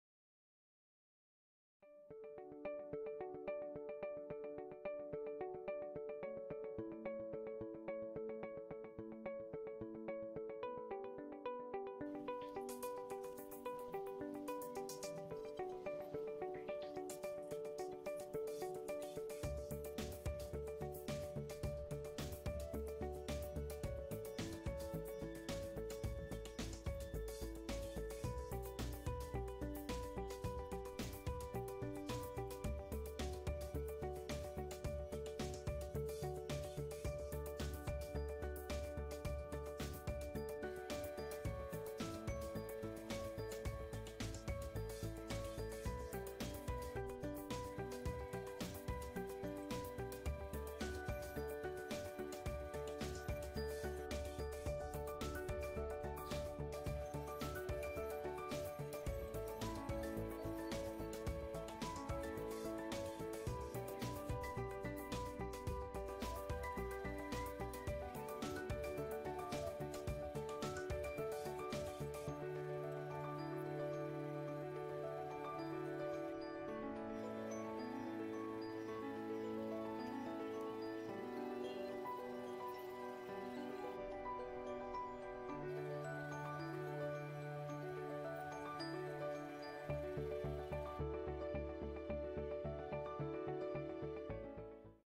Photoshop Smile Design .
Smile design using PHOTOSHOP to show patient possibilities for enhancing her smile .